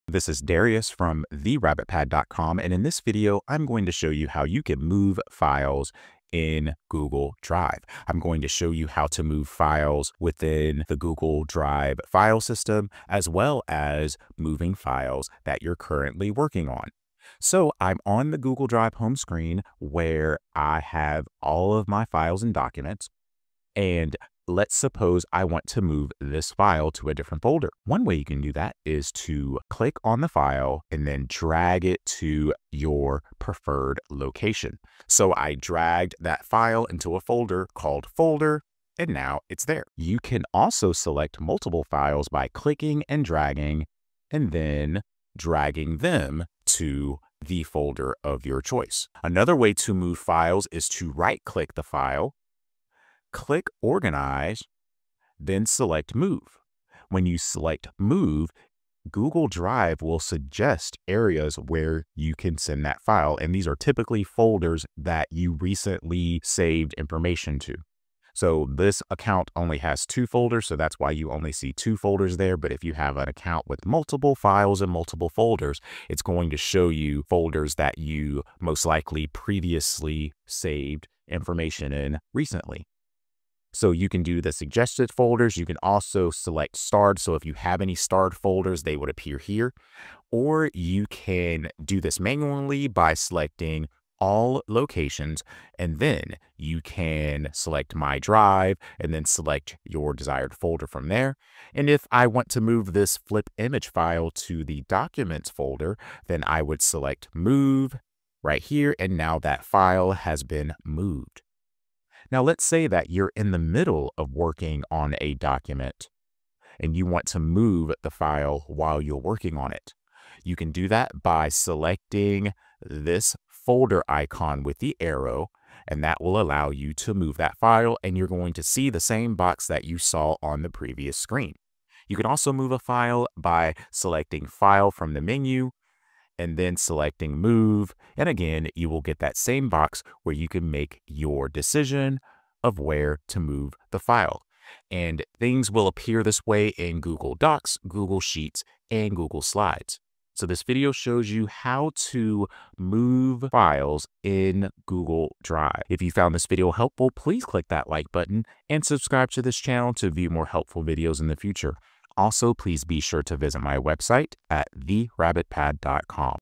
How to Move Files in Google Drive
Need to organize your Google Drive? In this quick tutorial, I’ll show you *how to move files and folders in Google Drive* so everything is where you need it. Whether you’re cleaning up your workspace or sorting shared files, moving files is simple and efficient once you know how.

You’ll learn how to:
✅ Move files using drag and drop
✅ Use the right-click menu to move files
✅ Organize your Drive like a pro

🎯 Perfect for students, professionals, or anyone with a messy Drive.